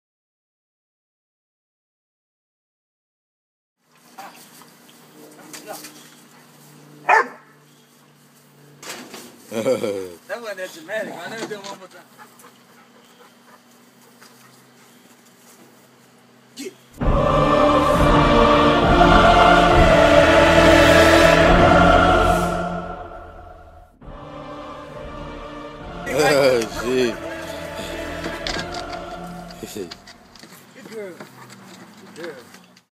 High Jumping Dog - Air bud - Football playing dog
Watch As my pit bull leaps into the air to catch the football. NFL prospect.
High jumping dog